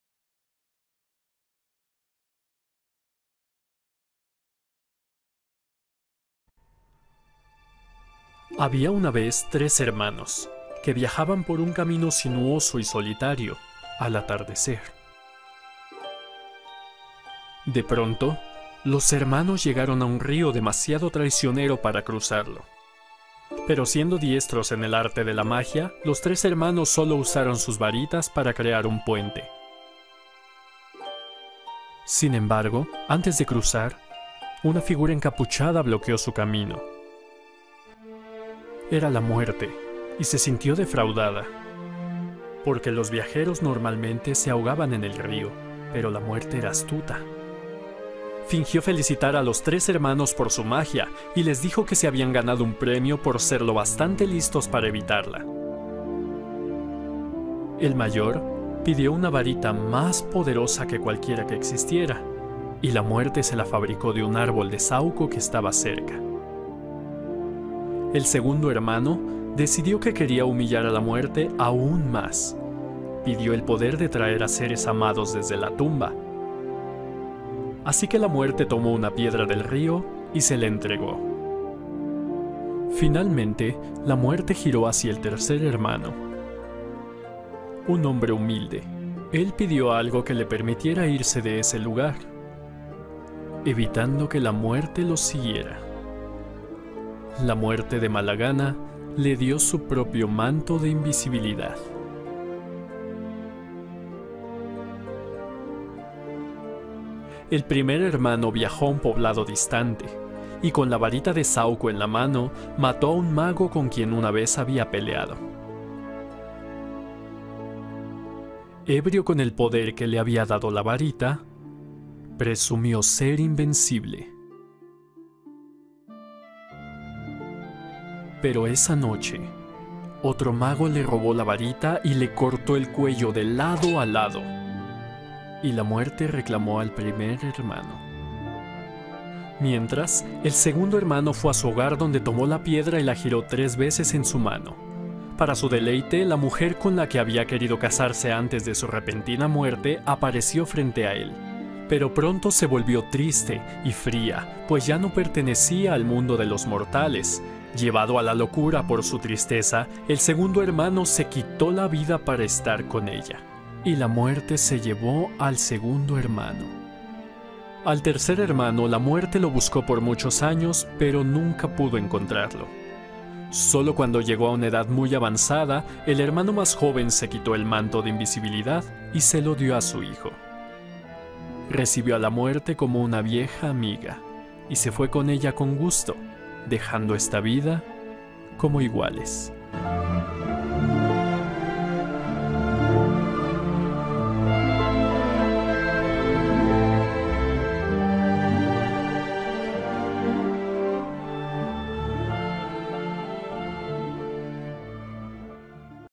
AUDIOCUENTO - Fábula de los Tres Hermanos (Harry Potter y Las Reliquias de la Muerte)
Ojalá les agrade esta narración que he realizado de esta maravillosa historia extraída del fascinante mundo mágico de Harry Potter.

Cuentos de Beedle El Bardo. Fábula de los Tres Hermanos.
El origen de las Reliquias de la Muerte.